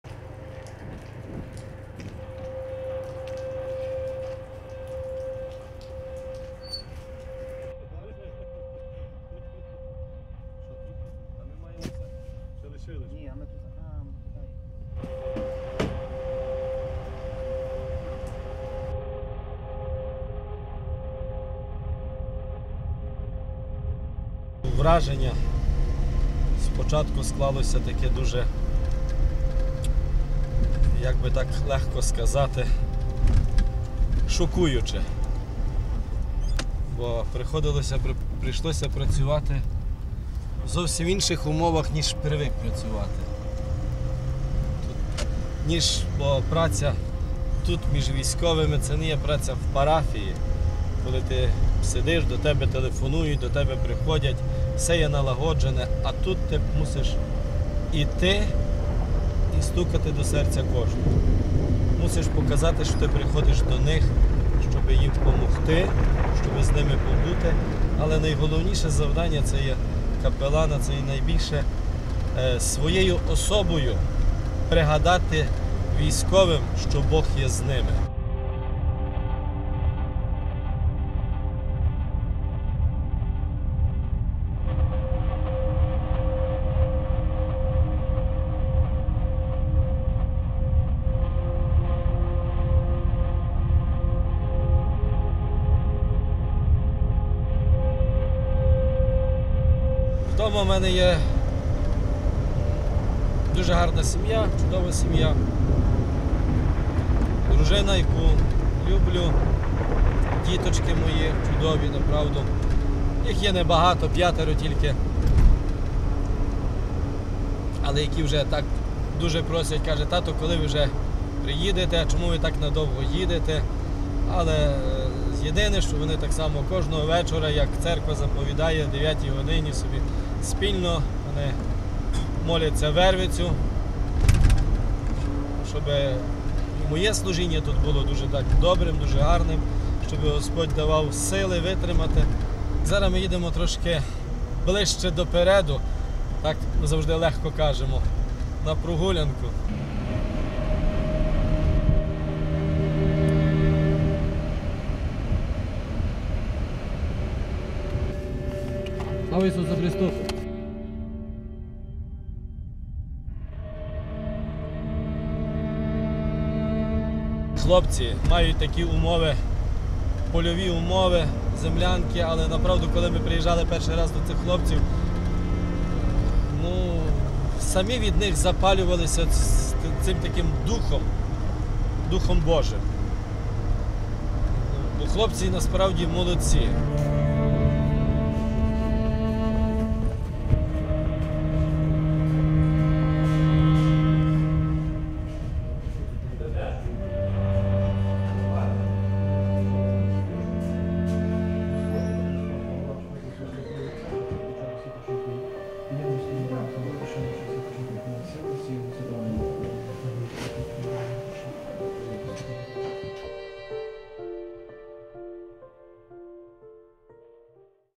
Капелан
Отець Богдан вже близько місяця перебуває у секторі М зони АТО: "Найголовніше завдання капелана - своєю особою пригадати військовим, що Бог є з ними". Разом з отцем ми проїхали на передову, щоб відправити літургію, посповідати військових та підтримати наших хлопців.